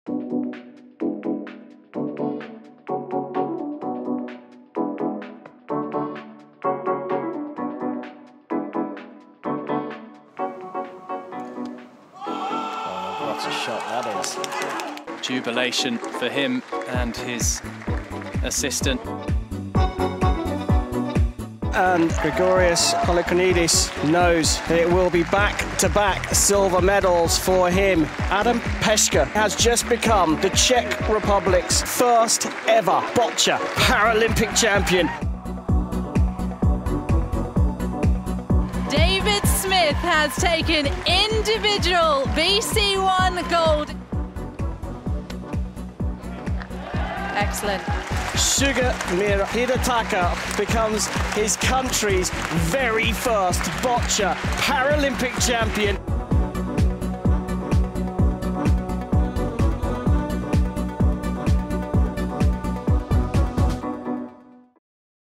Boccia Highlights | Day 8 | Tokyo 2020 Paralympic Games
What a day for Boccia at the Paralympic Games! Sloviakia's Samuel Andrejcik was unstoppable, winning 4-0 against Pornchok Larpyen!